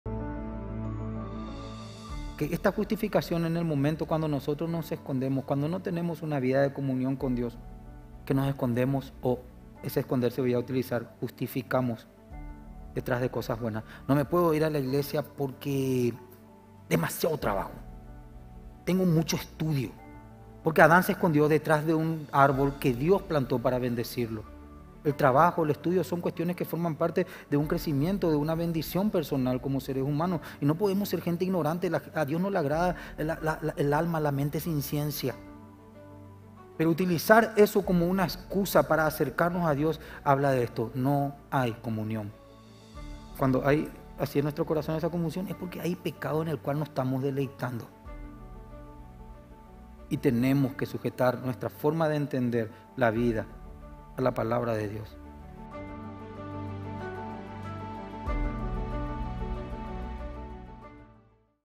MDI295 Minuto de Impacto MQV - Justificados detrás de las cosas buenas
¿Estás constantemente poniendo pretextos para acercarte más a Dios? Aún las cosas más buenas de la vida, si no están ordenadas debidamente, pueden ser excusas que nos vayan alejando poco a poco del Señor.
¡No más justificaciones! Busquémoslo hoy.

➡️ Fragmento de la prédica "Enséñanos a orar" del Pr.